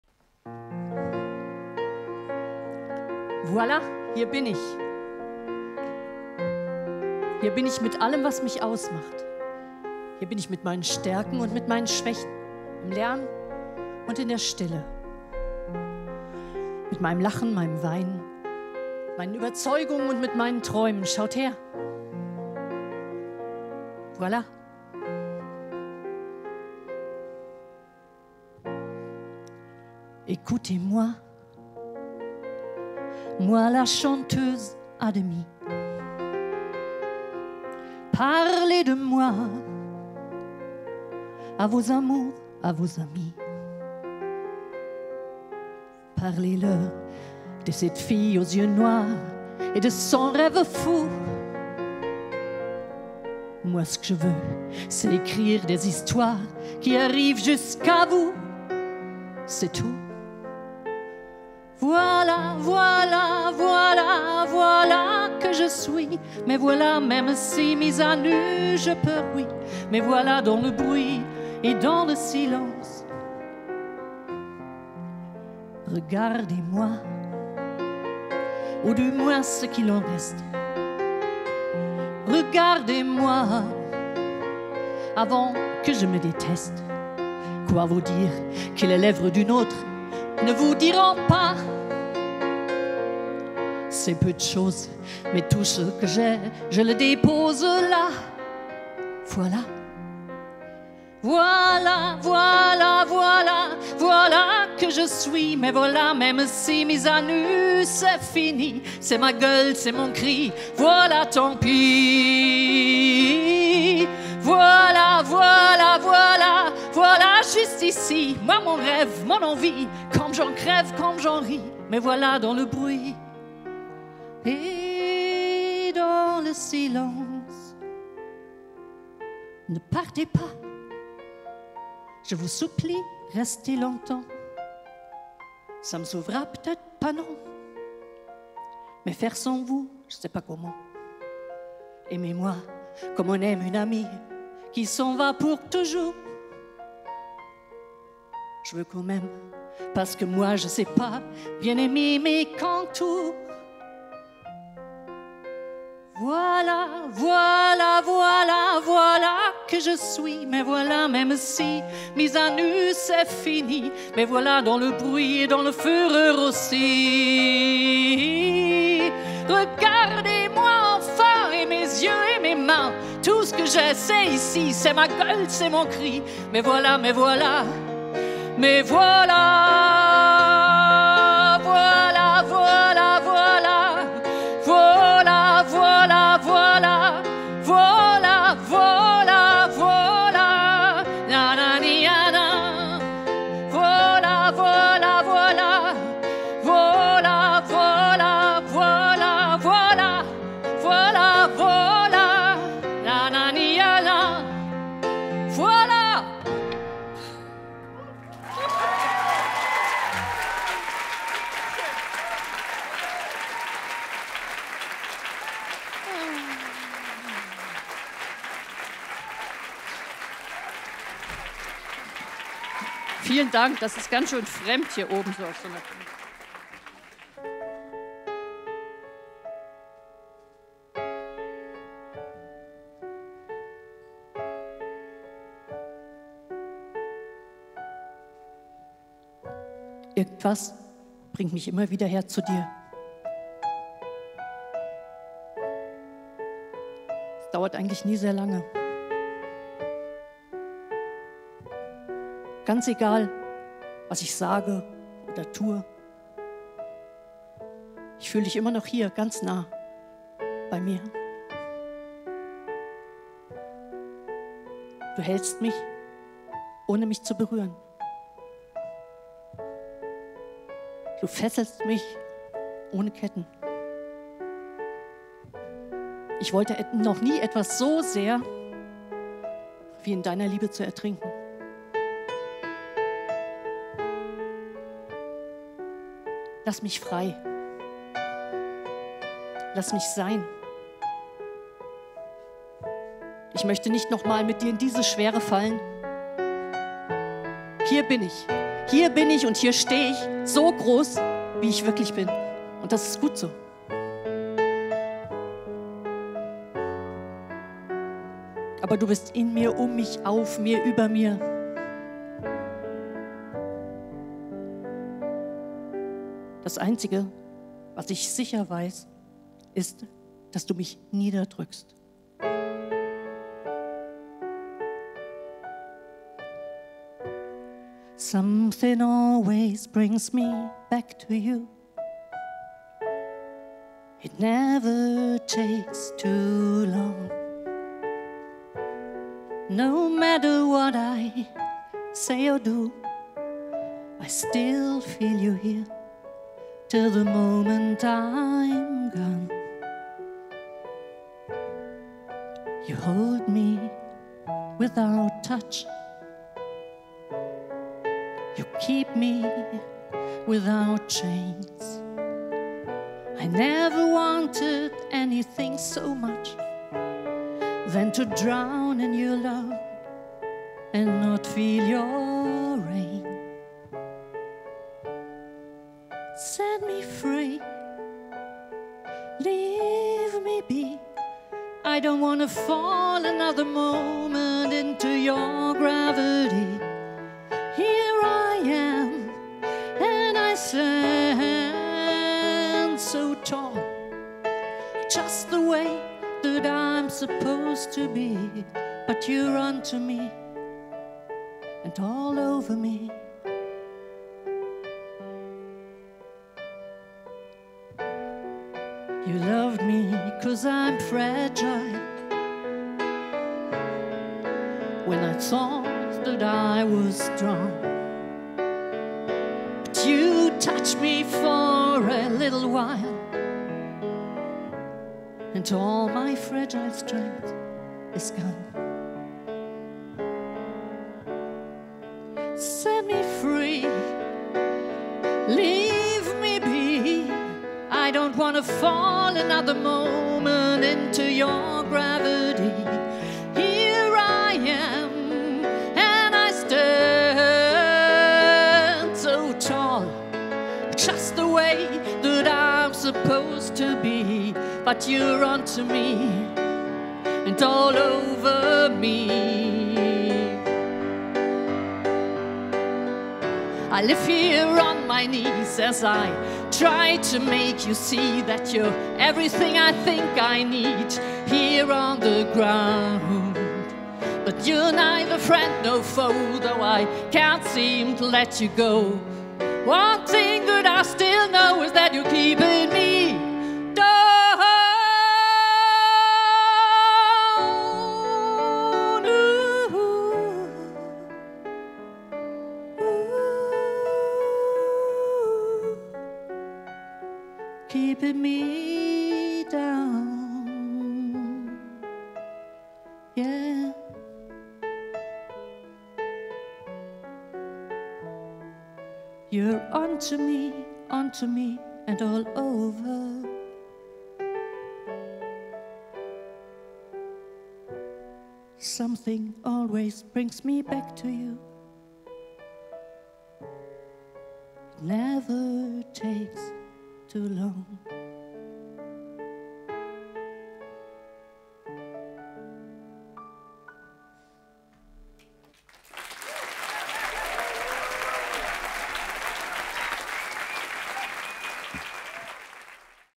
327. Marburger Abend: Sabine - Klavier & Gesang
Sabine mit Klavier und Gesang beim 327. marburgerabend, der mutteralleropenstages im kulturzentrum